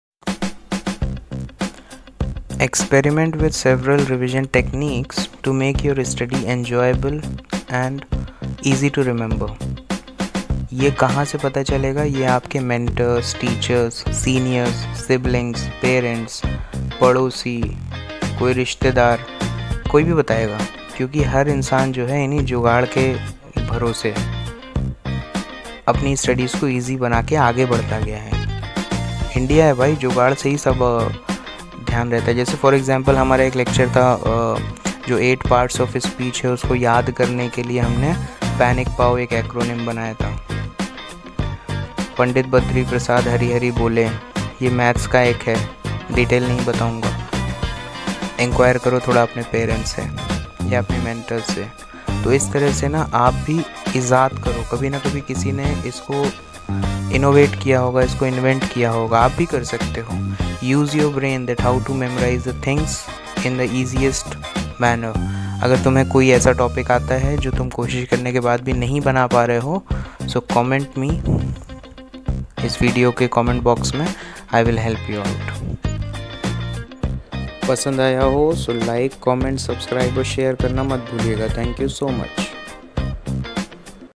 Exam Tip 9 | Memorising Techniques | Indian जुगाड़ | Still Time To Prepare For Exams
Follow these tips to score well in the exam and to motivate yourself.